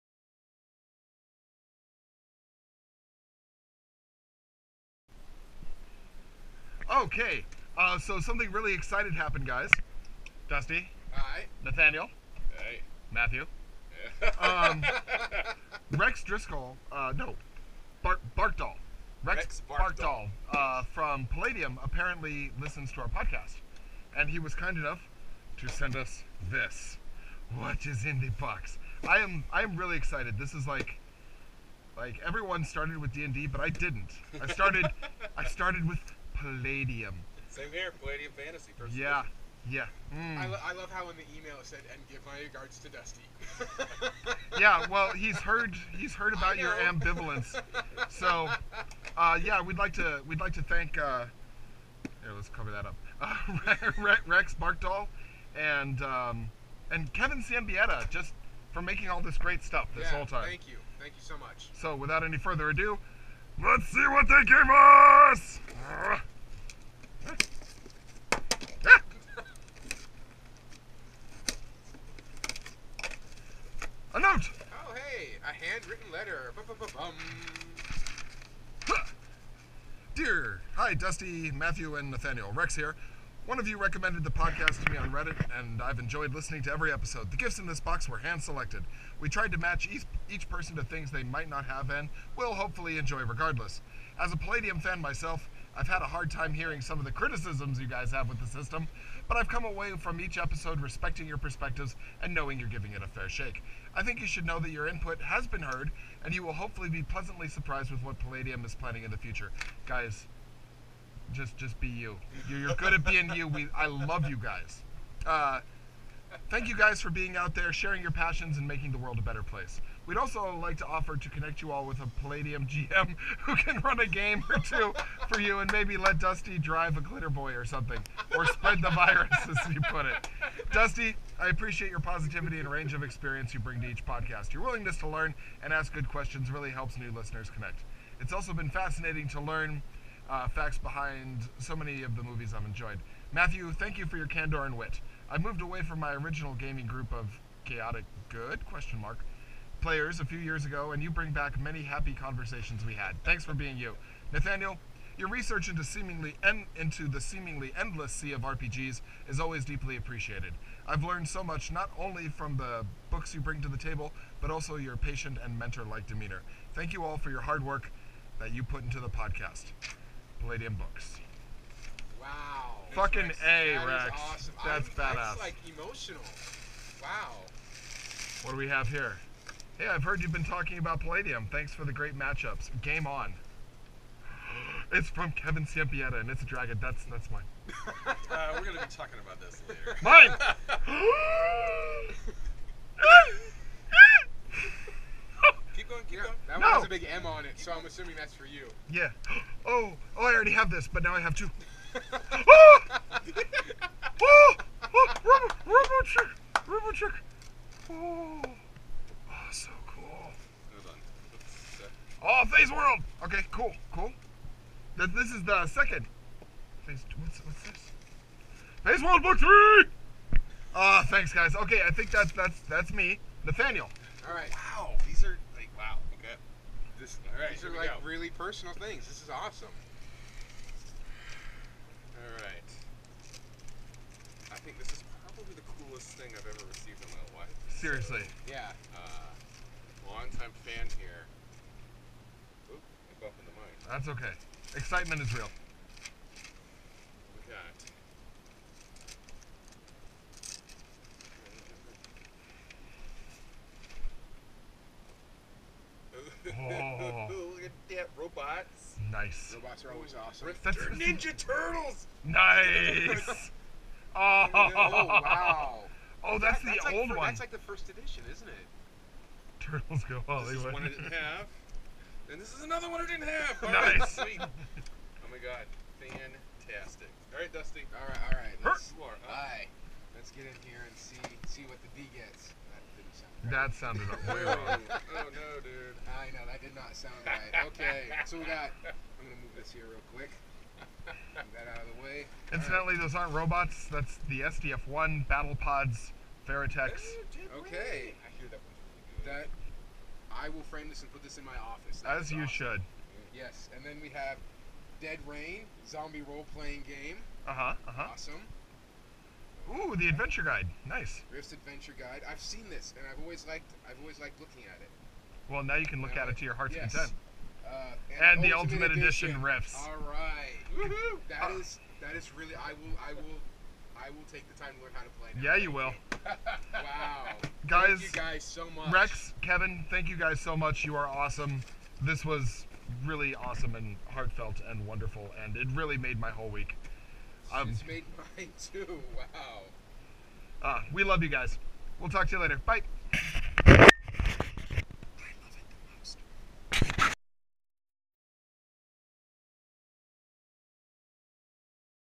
Palladium Books Care Package Unboxing
Hey folks! I'm in the process of catching up with a massive backlog of video uploads. I wanted to kick off this new load of content with a special unboxing video we took of a really frickin sweet care pacxakgfe that Palladium Books sent us a few months back!

SO heads up: this camera was mounted on Matthew's head and he was very very very excited. So there's a lot of shaky cam going on, and we apologize if our ridiculous excitement causes nausea!

And also, please pay no need to the messy state of the environs, this was right before we moved out of the old studio, and a lot of packing was being done.

Anyway, once again, to our friends at Palladium Books, we say thanks for the awesome words and gifts.